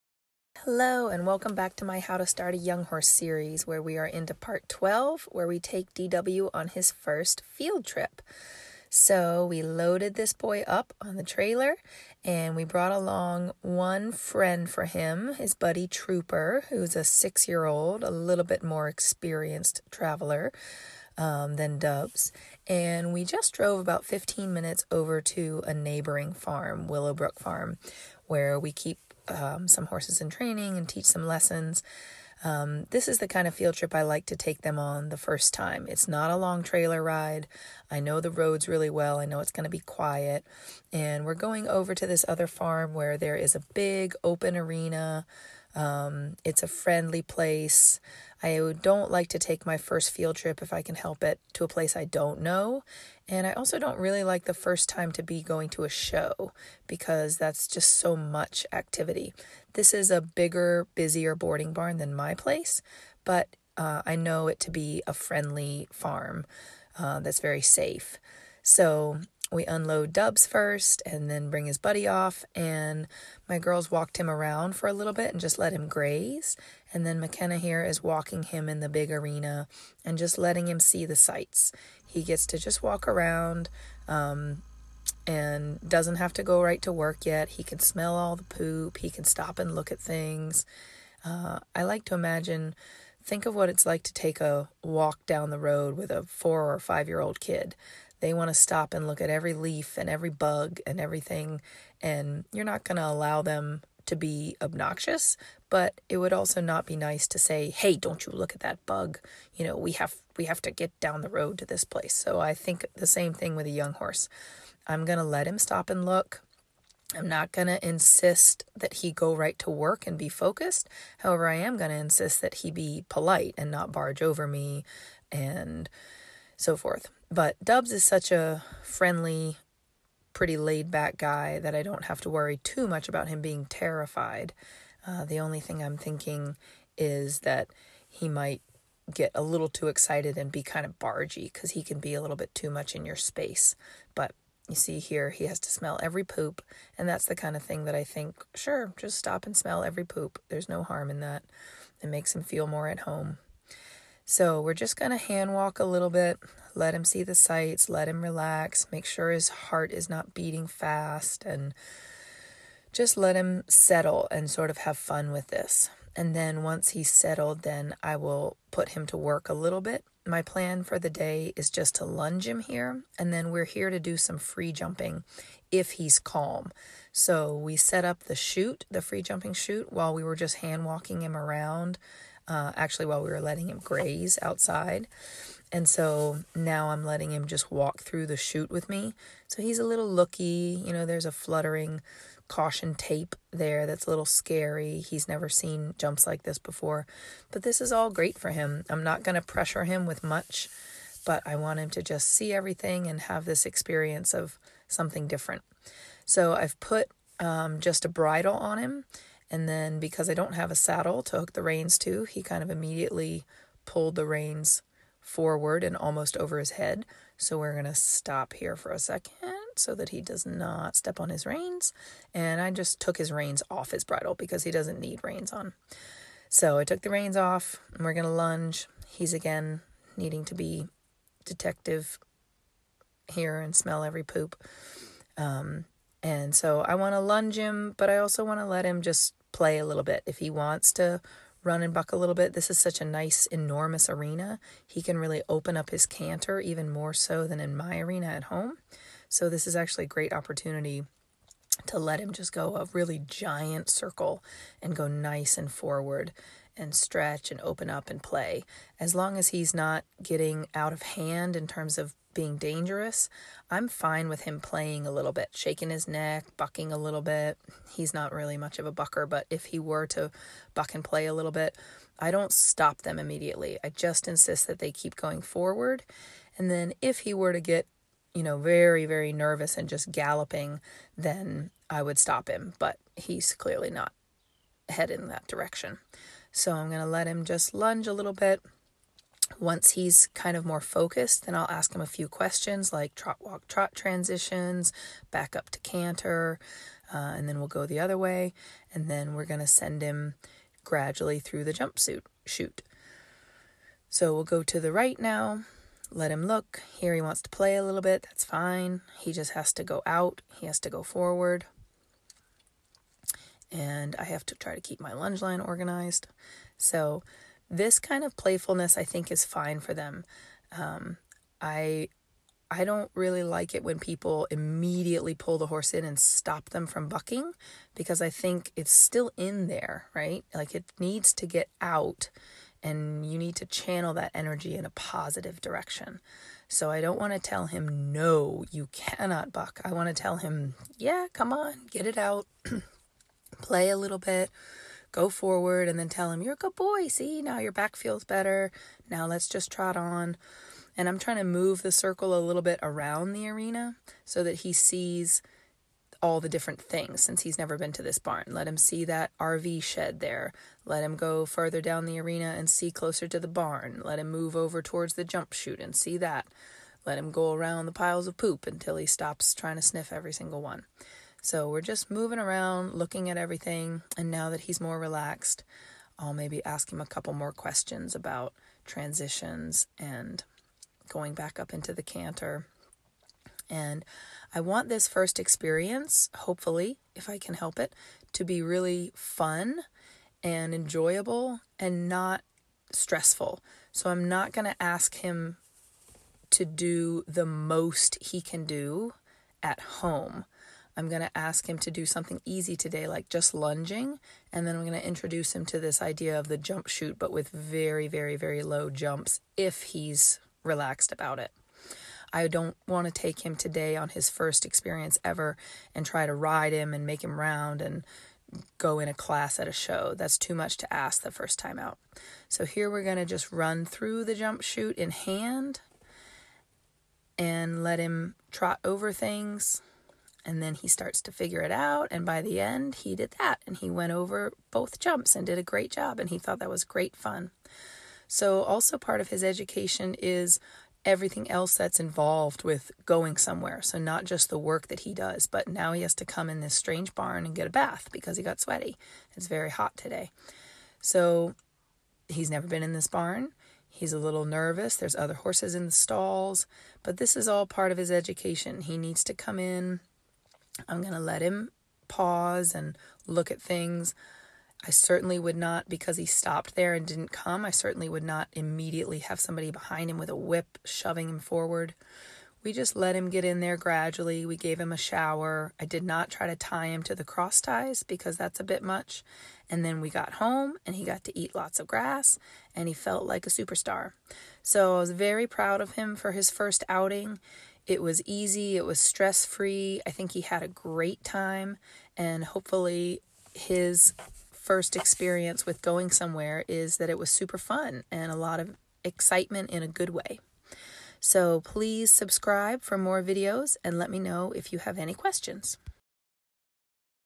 How To Start A Young Horse - Part 12 - First Field Trip!
DW’s first field trip off the farm! I talk about how I prepare the first off site adventure. How to ensure (as well as anyone can) that the young horse has a positive experience with little stress and a lot of fun!